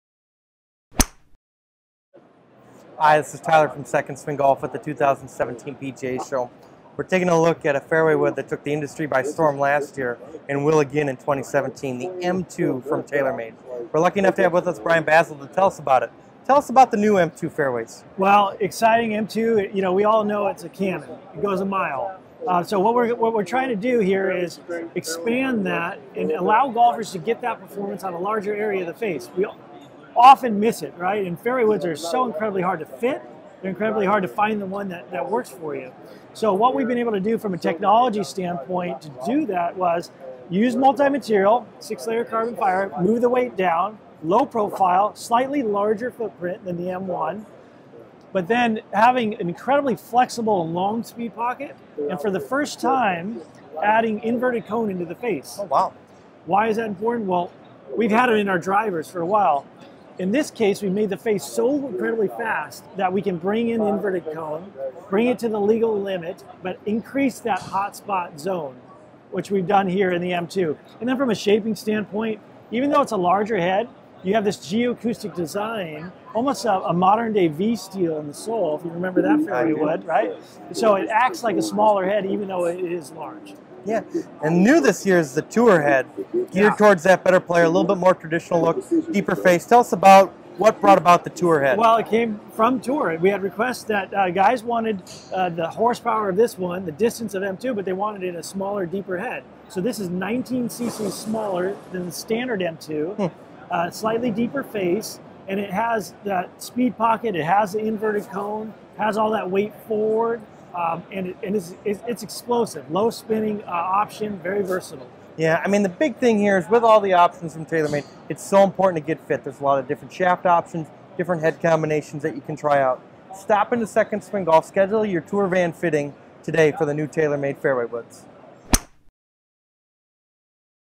TaylorMade M2 Fairway Review
Building off their popular model from a year ago, TaylorMade introduces and even better version of the M2 and M2 Tour Fairway Woods. Brian Bazzel the Senior Director of Product Creation talks about the changes.